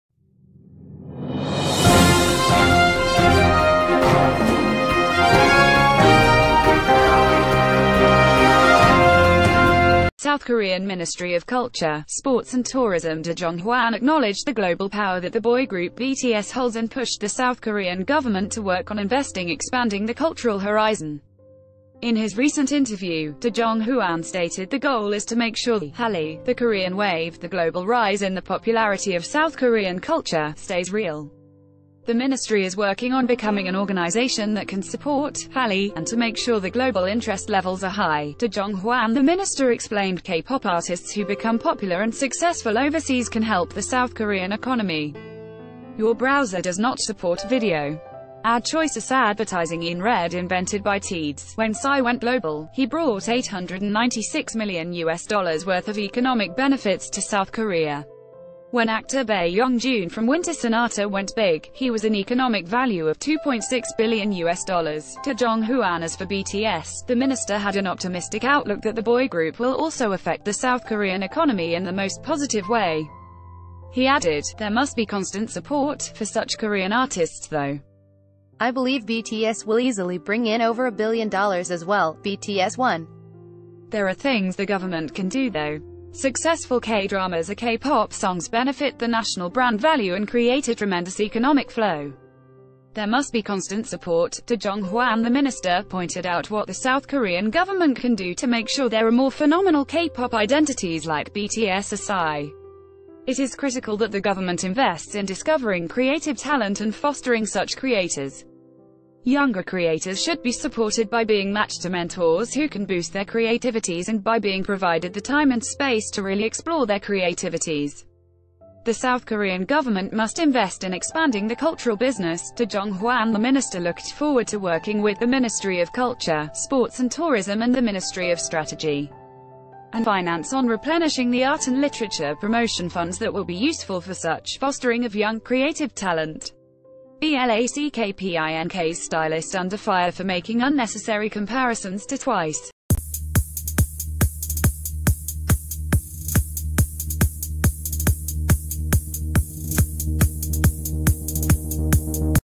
Government Official Says Korea Should Do More To Recognize BTS’s Global Impact
South Korean Ministry of Culture, Sports and Tourism Do Jong Hwan acknowledged the global power that...